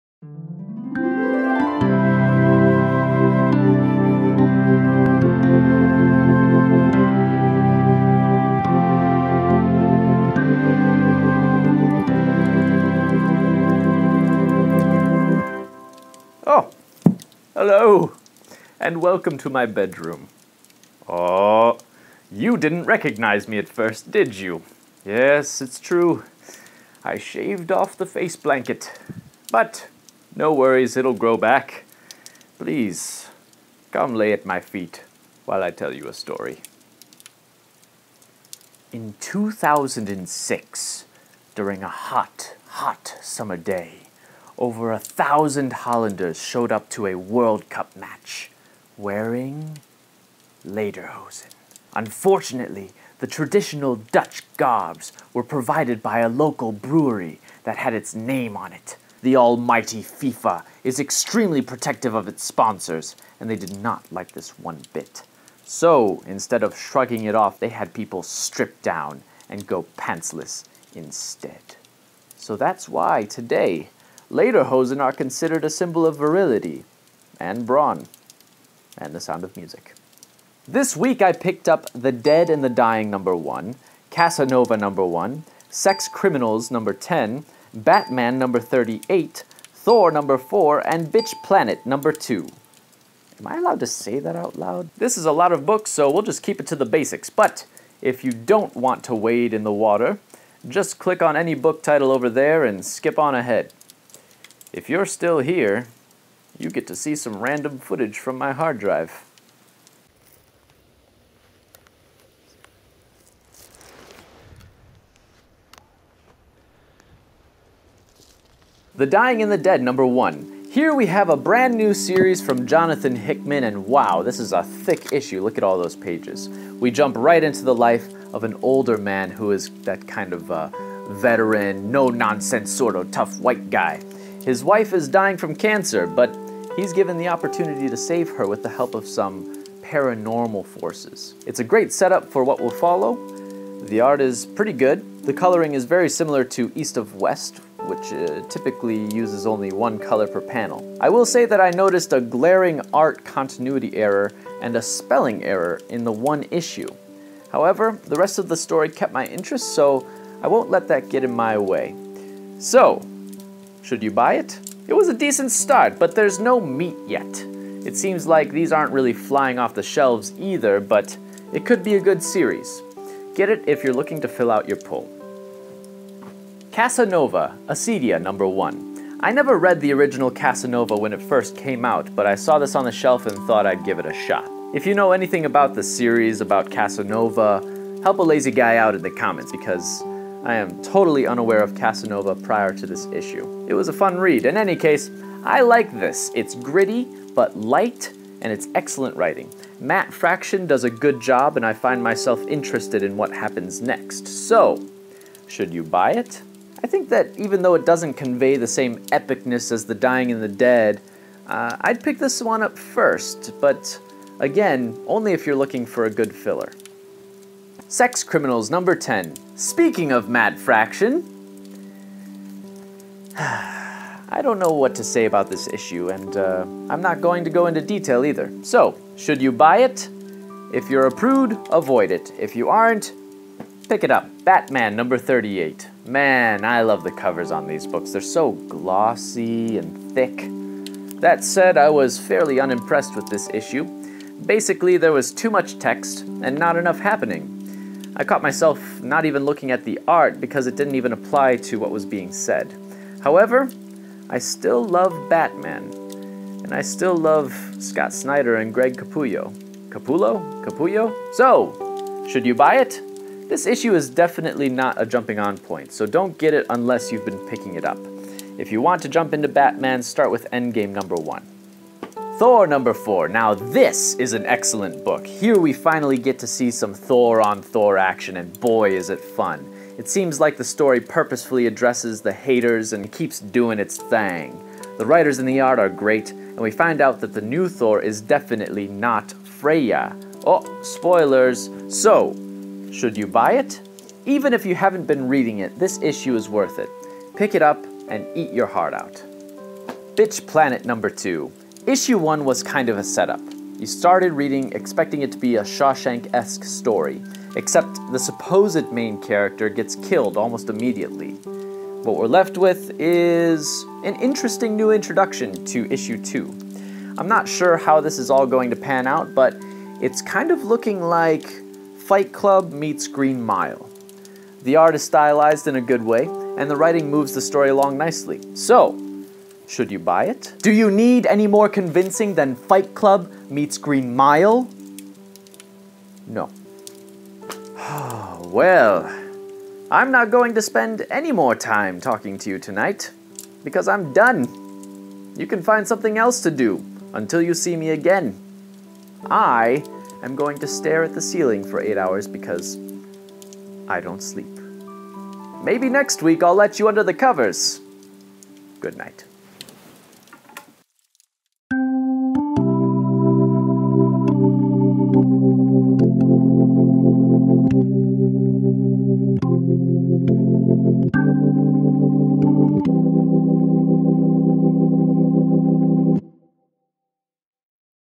Bedside Comics 4: Lederhosen
Shout out to all the Dutch people out there!  Thanks for gifting the world with wedgie pants.  A lot of books this week, so my thoughts are short and pointed...like a dart.  This week, we're discussing:

The Dead & The Dying #1
Casanova: Acedia #1
Sex Criminals #10
Batman #38
Thor #4
Bitch Planet #2

Leave your own thoughts below and feel free to give suggestions for books you'd like me to pull for next week's show.  Thanks!

This video was posted for comics released on 1/28/2015.

All other content is original and is the creative property of 123comicbro.